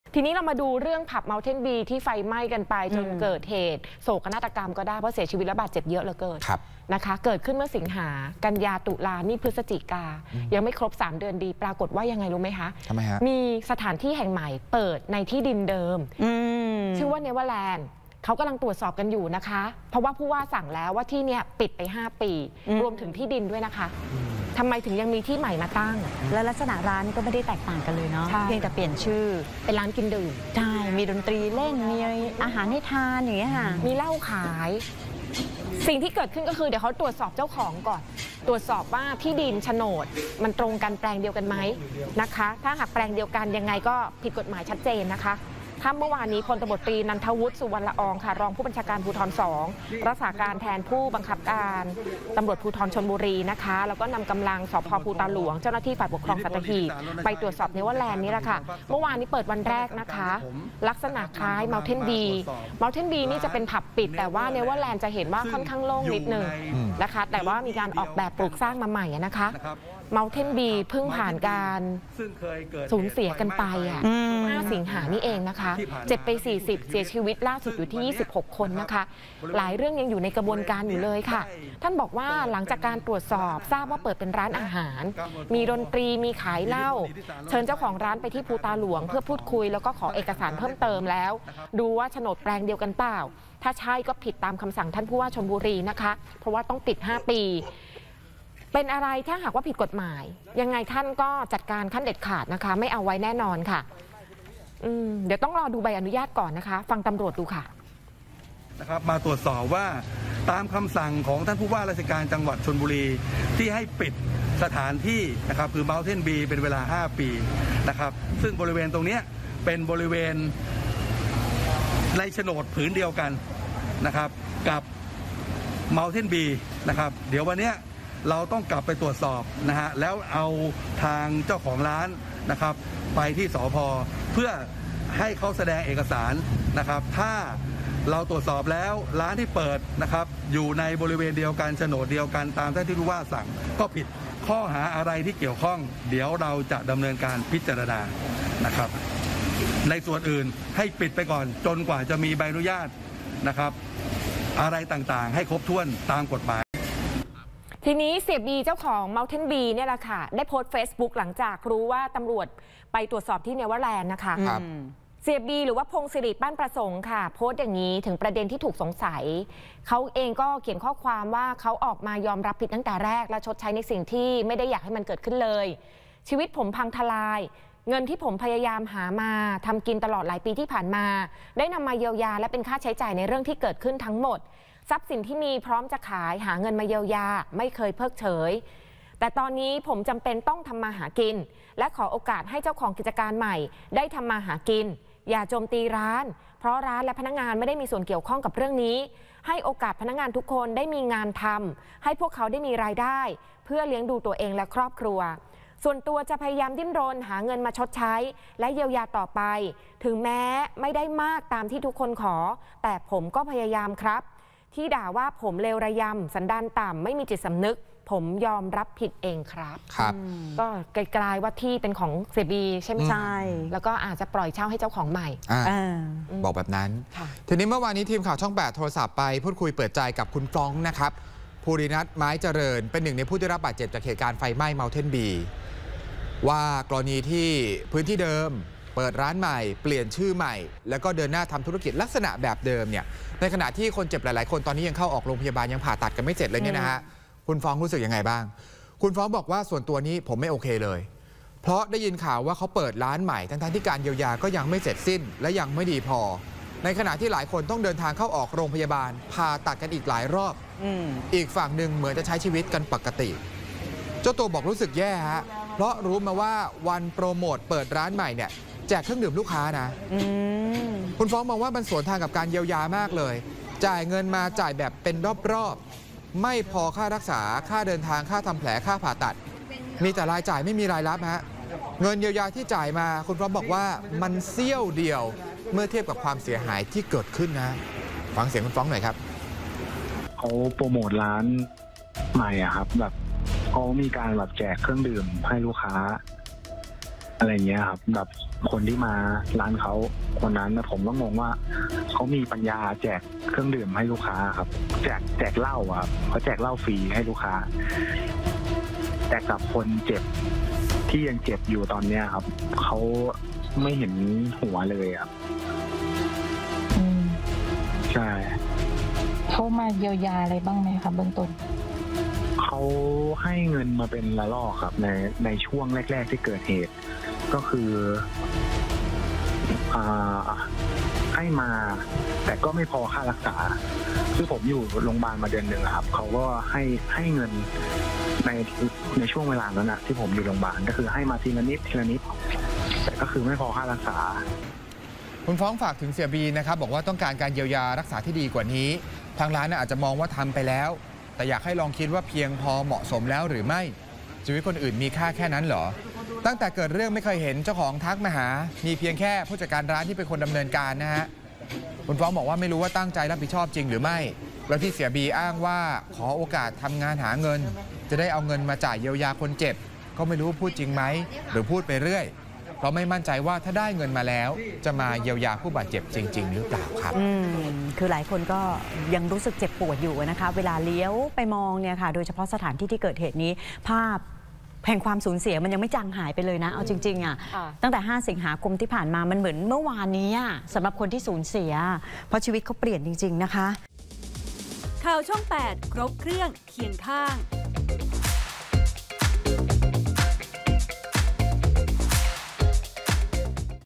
สั่งปิดร้านเหล้าใหม่ สร้างทับที่เมาน์เทนบี | ข่าวช่อง8
ตำรวจบุกจับสถานบันเทิงที่เพิ่งเปิดใหม่วันแรก ที่ตั้งเดียวกับเมาน์เทนบี ผับมรณะ 26 ศพ เชิญเจ้าของร้านแจงเปิดได้อย่างไร เพราะอยู่บนโฉนดผืนเดียวกัน ที่มีคำสั่งปิดเป็นเวลา 5 ปี